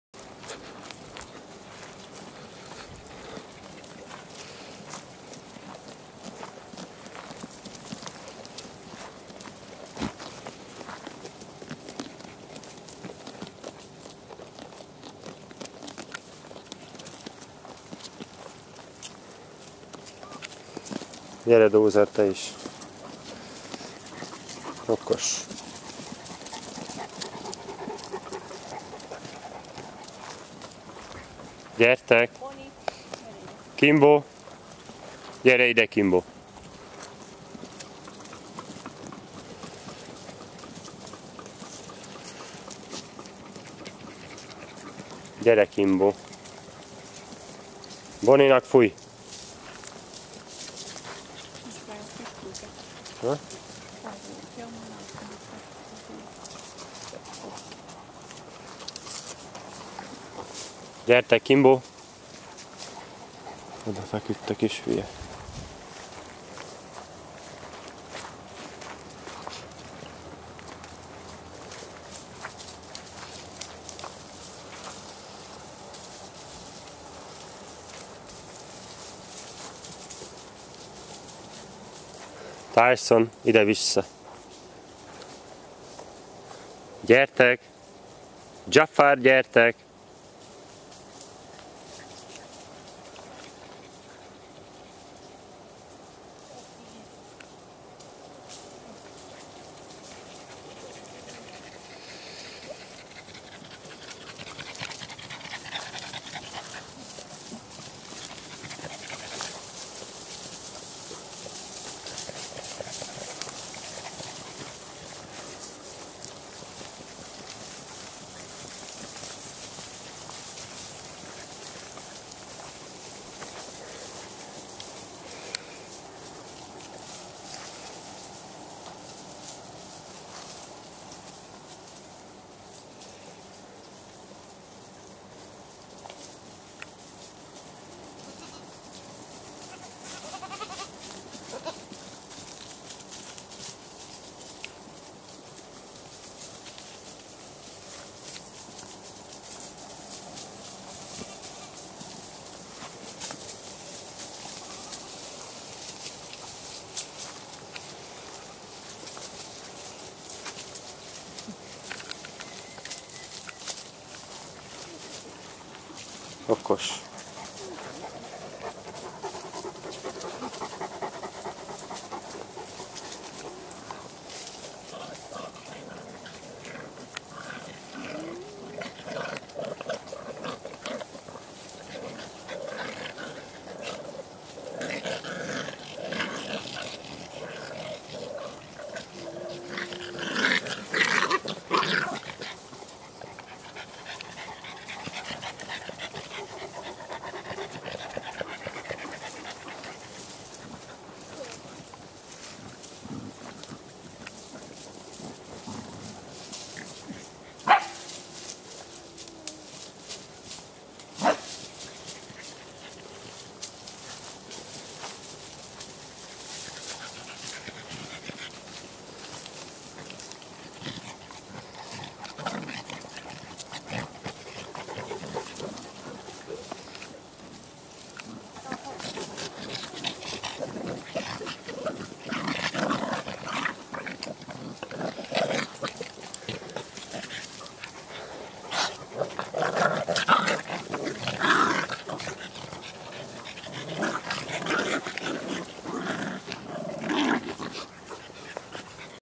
KutyaFarm 3.Rész... :)
9 Kutya, 8 Kecske, Larina&Mónika, és aki mindenkit irányít, mindenkit felügyel, mindenkit kordában tart, a Wezérbika: Zander Greg!!!  :)